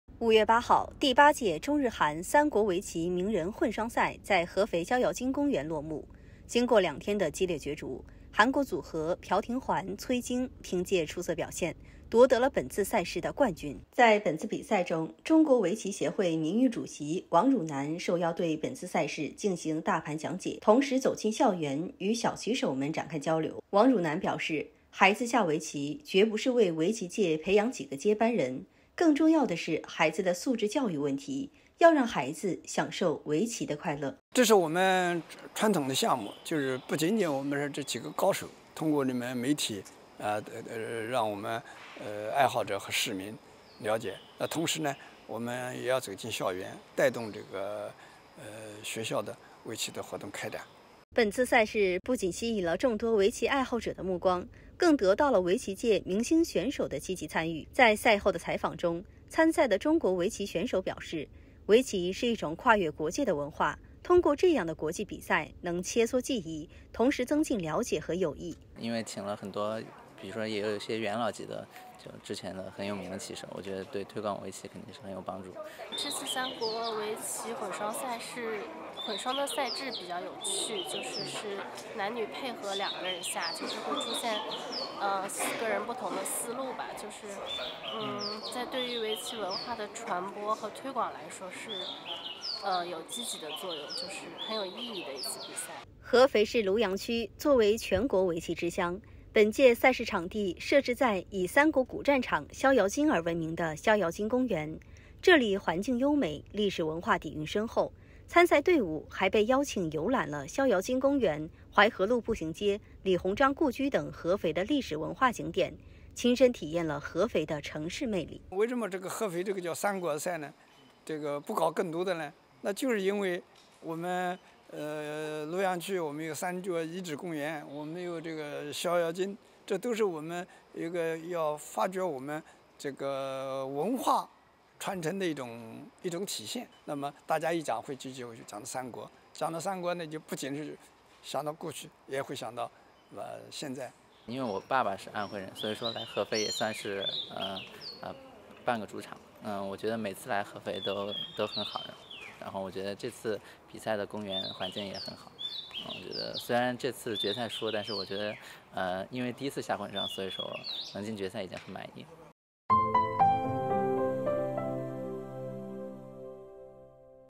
中日韩三国围棋手切磋棋艺 推广围棋文化
第八届中日韩三国围棋名人混双赛5月7日至8日在安徽省合肥市举行，该项混双赛吸引了中日韩三个国家16位明星选手参赛。经过两天的激烈角逐，韩国组合朴廷桓/崔精凭借出色表现，夺得了本次赛事的冠军。

在本次比赛中，中国围棋协会名誉主席王汝南受邀对进行大盘讲解，同时走进校园，与小棋手们展开交流。王汝南表示，孩子下围棋，绝不是为围棋界培养几个接班人，更重要的是孩子的素质教育问题，要让孩子享受围棋的快乐。